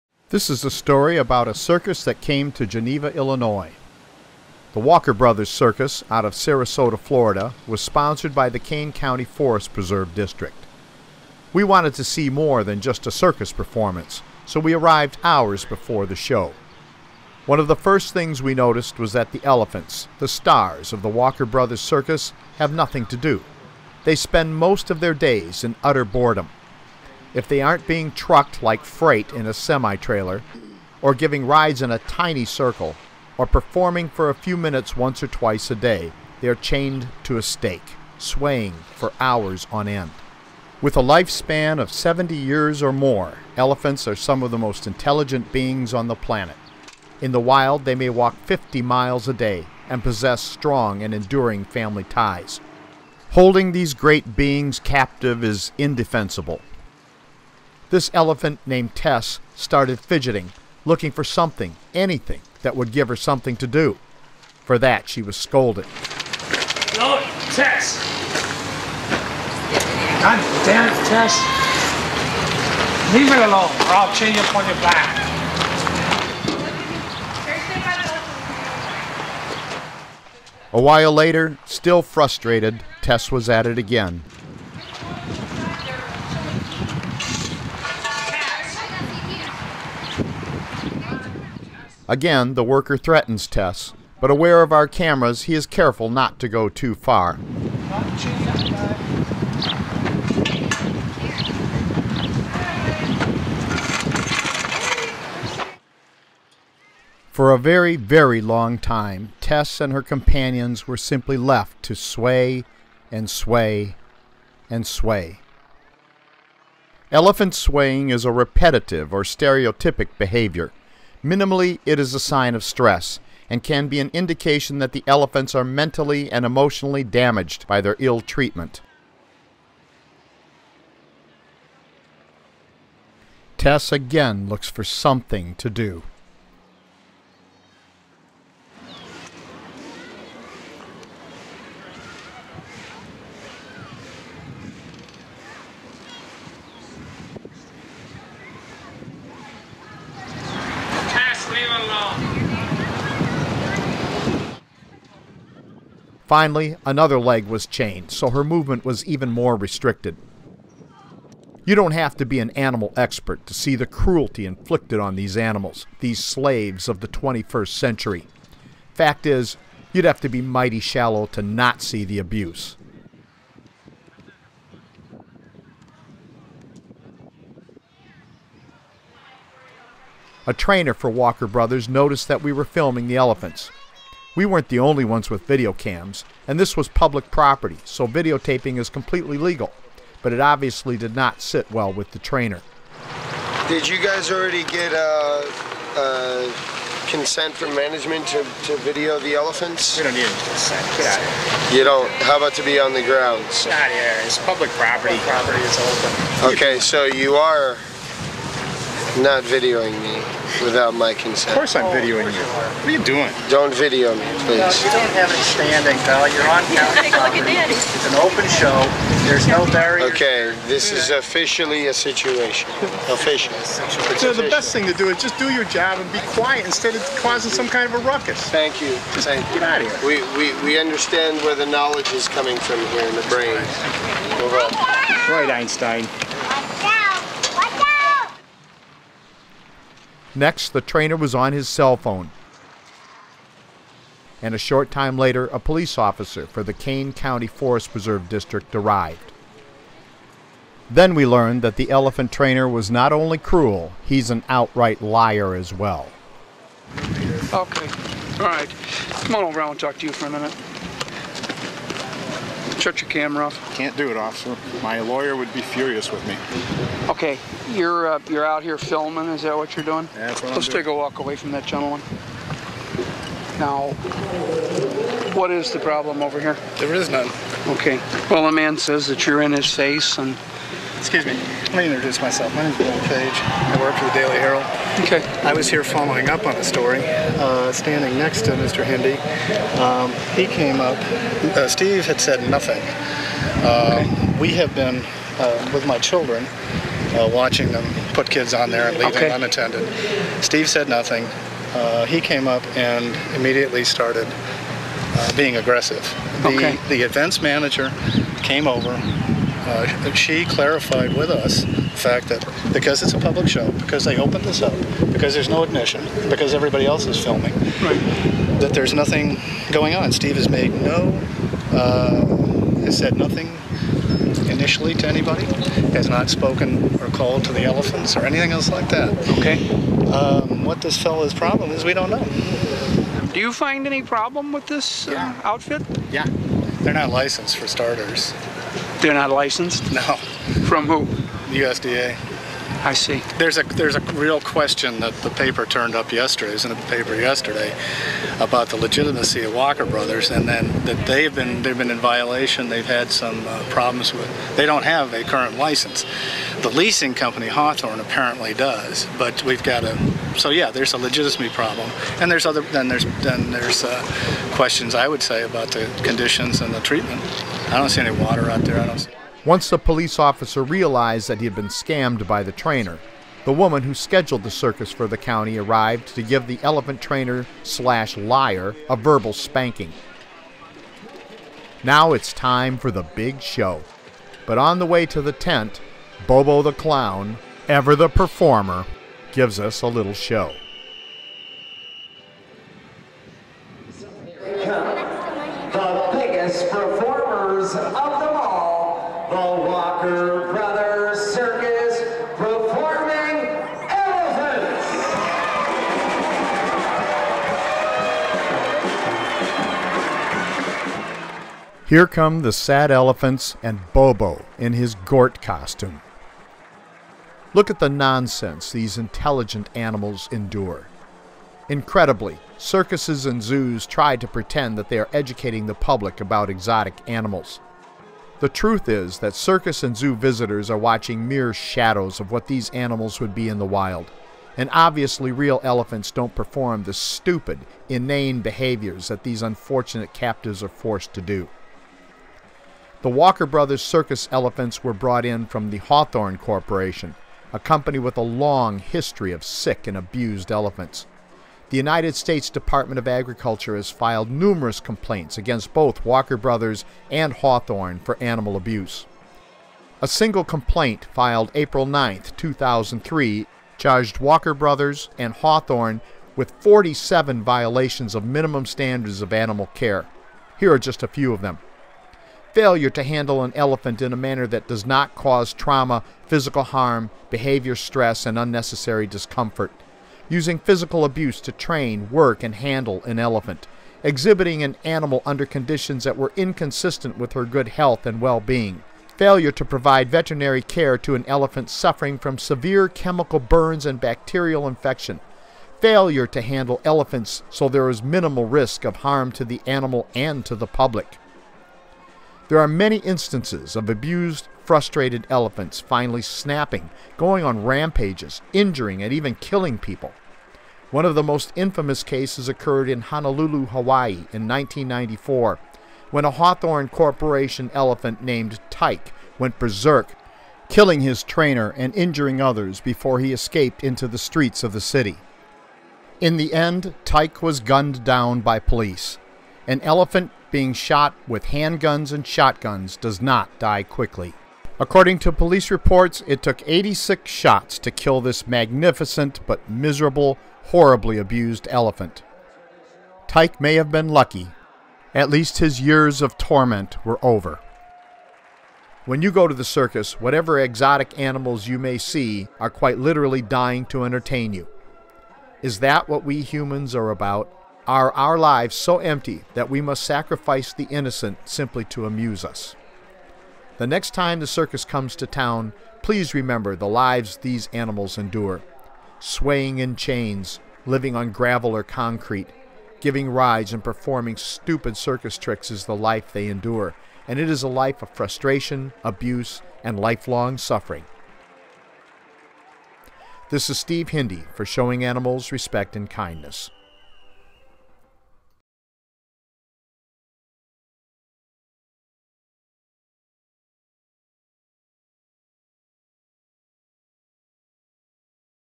Cruelty to Circus Elephants
The Walker Brothers Circus has a history of abuse to elephants and other animals unfortunate enough to be in its care.  The United States Department of Agriculture has repeatedly charged the Walker Brothers Circus with a multitude of violations.  This videos shows the kind of abusive and untrustworthy people who are part of the circus industry.